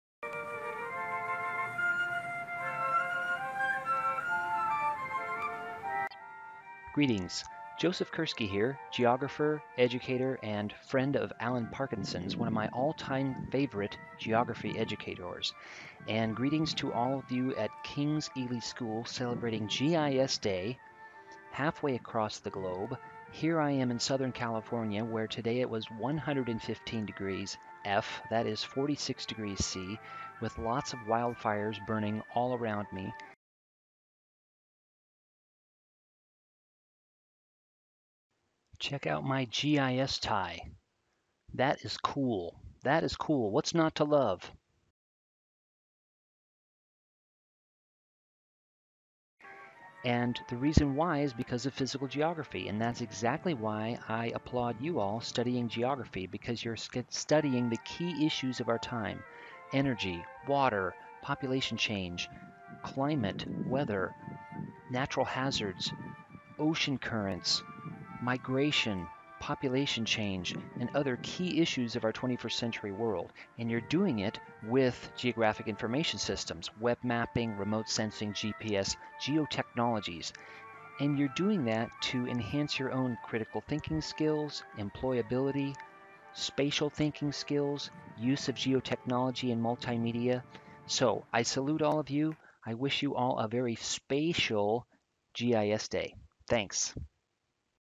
GIS Day Greetings to King's Ely School
GIS Day Greetings to King's Ely School and Alan Parkinson, one of my favorite geography educators.  Students, keep on studying GIS!